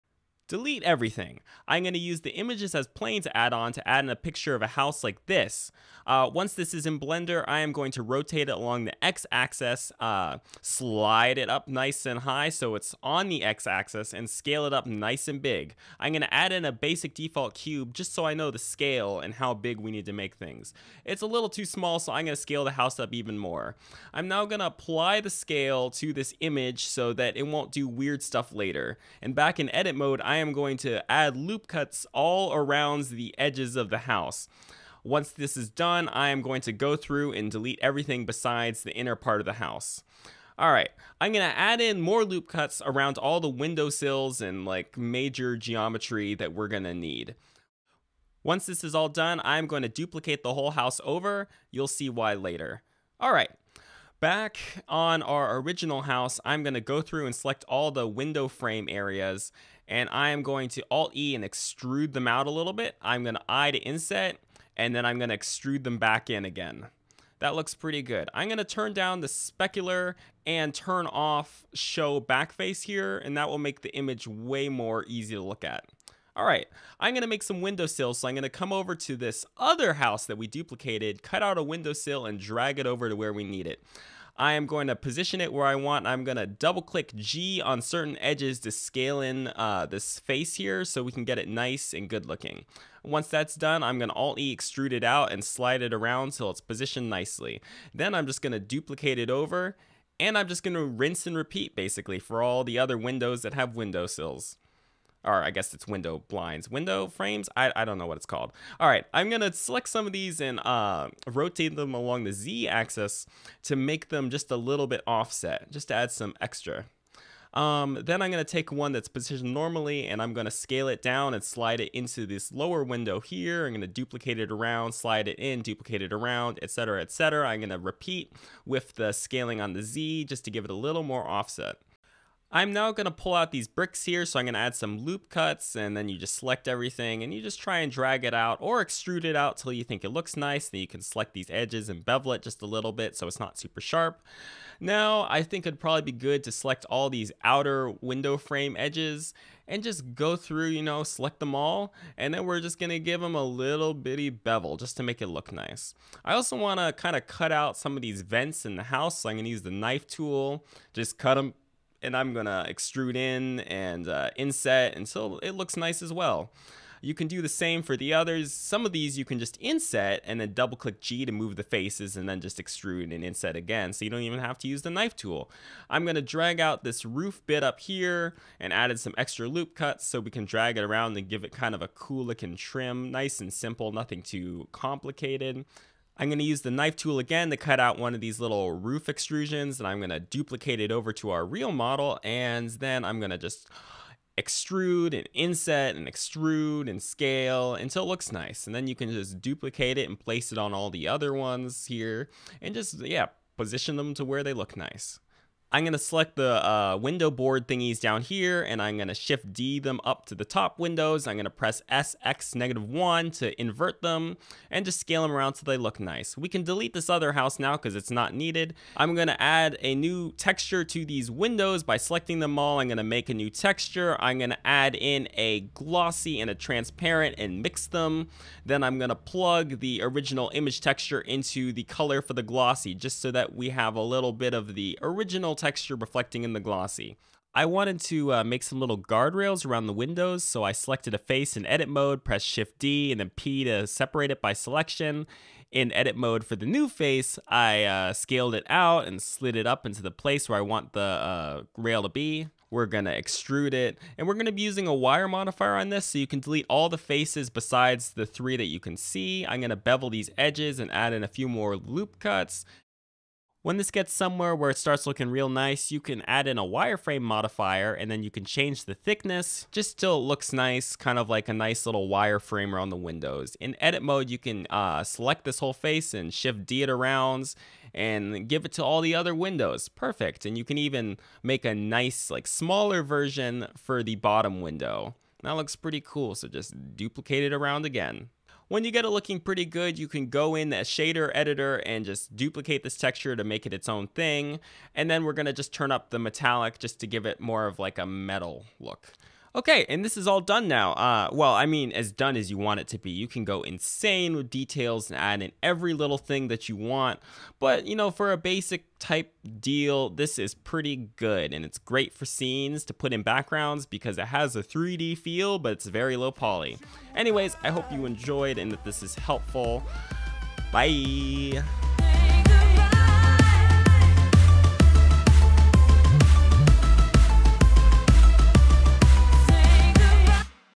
Making a (Fast n' Easy) Building | Blender 3.0 tutorial
Hey there, y'all! Comin' at you right now with another short tutorial on how to make some easy building props in Blender. Hope this is helpful!

00:00 Delete EVERYTHING and Let's Get Started!
00:26 Getting Loopy
00:48 Makin' some Windows
01:14 Window Boards
02:11 House Keeping and Lil Details
03:38 Window Work
03:57 Fancy Guard-Rails and Wireframe Modifiers
04:59 Outro!